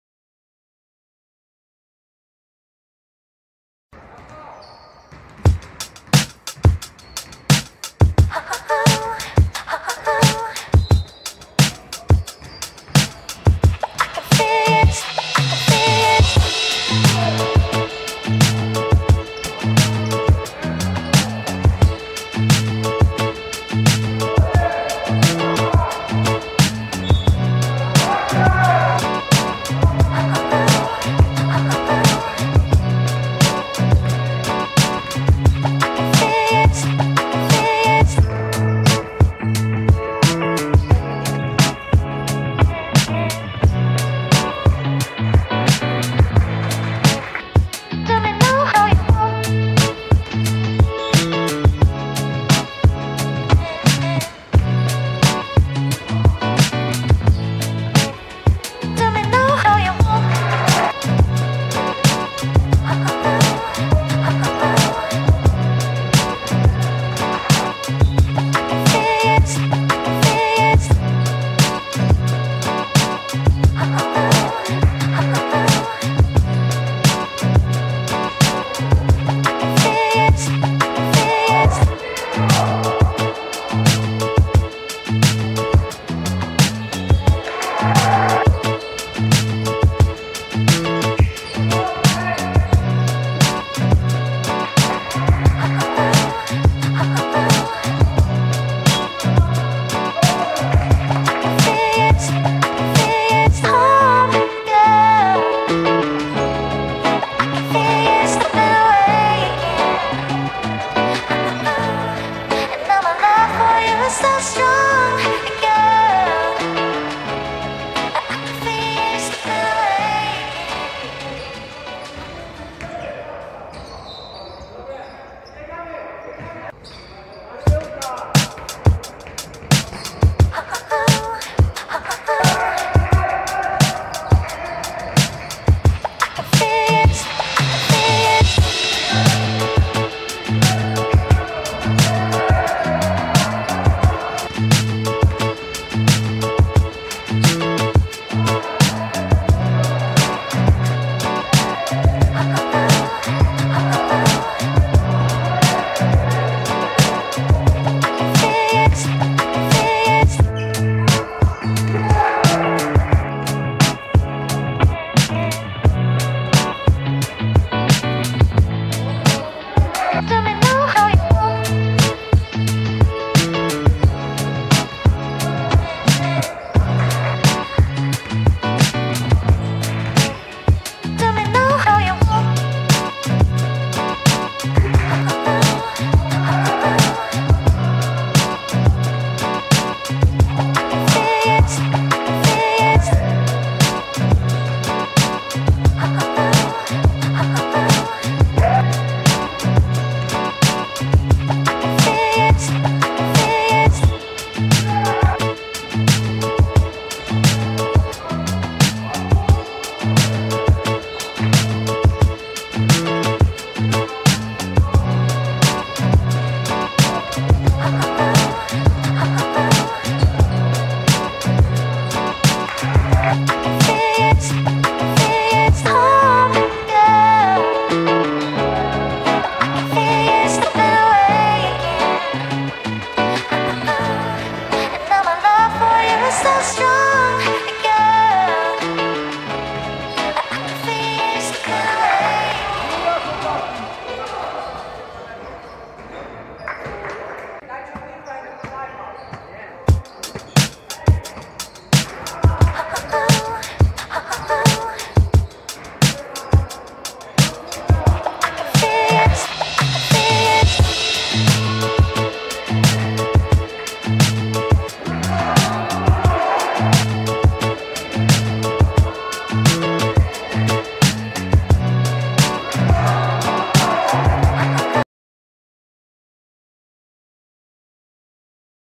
04 03 Villanova vs Davidson – Division 2 Zima 2022 23
Villanova 55:52 Davidson
J.Norman 31pkt 7-8ft 10zb 3ast 4stl
M.Koszowski 21pkt 8zb